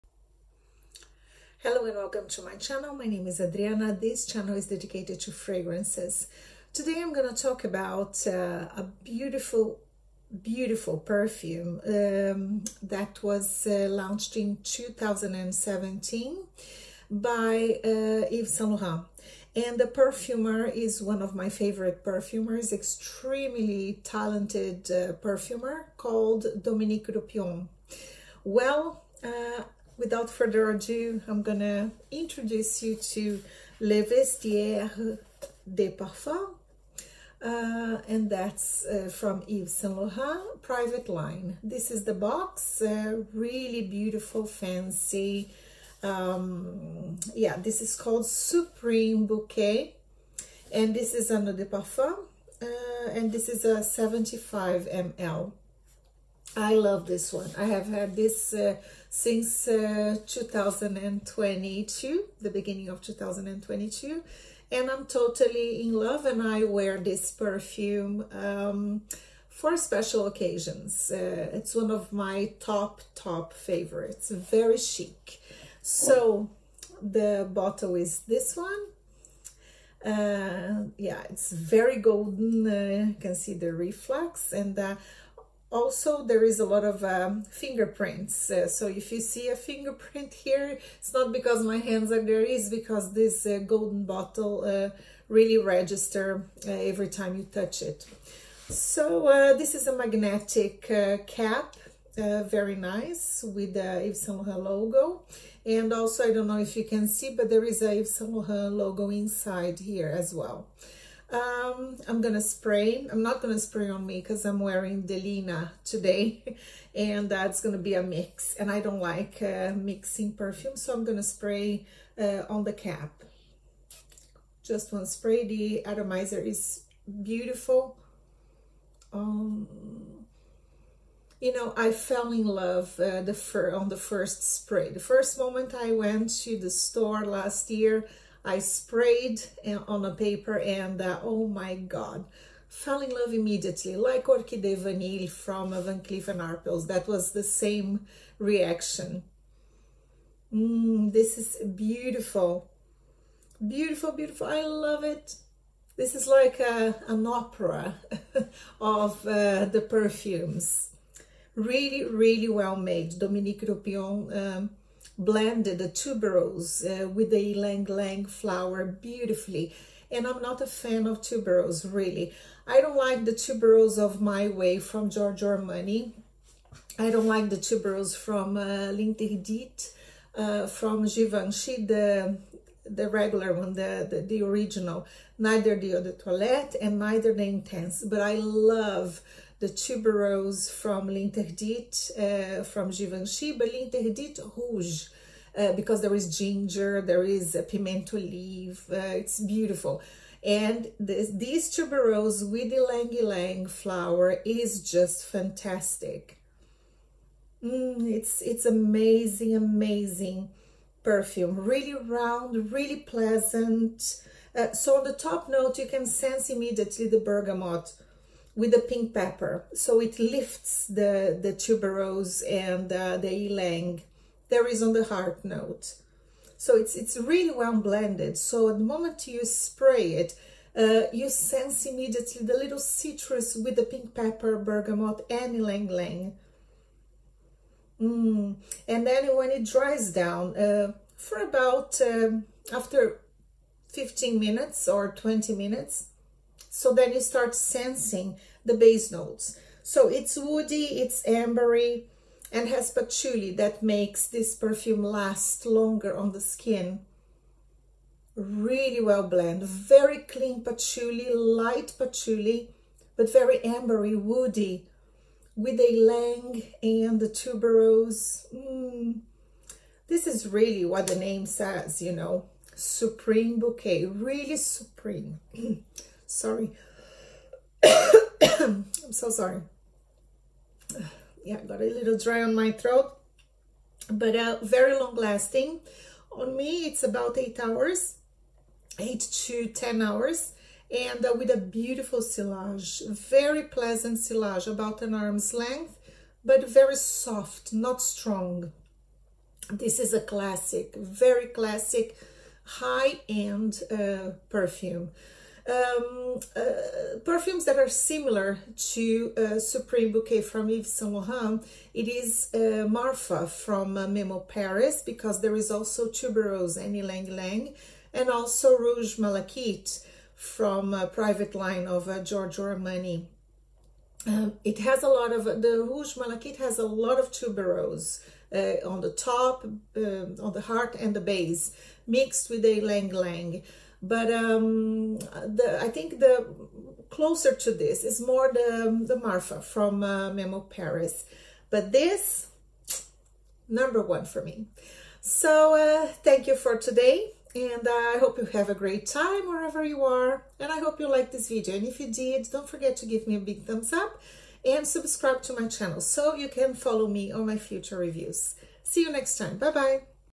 Supreme Bouquet edp Le Vestiaire des Parfums YSL
Yves Saint Laurent has a fantastic private line of perfumes, all of them are very well made by well accomplished perfumers. Supreme Bouquet was a love at first sniff for me last year, I had to have it 😍 in my little collection❤️.
It is one of my top top fragrances for special occasions 🌺
The Tuberose is amazing on this one and I do not enjoy that bubble gum tutti fruity tuberose.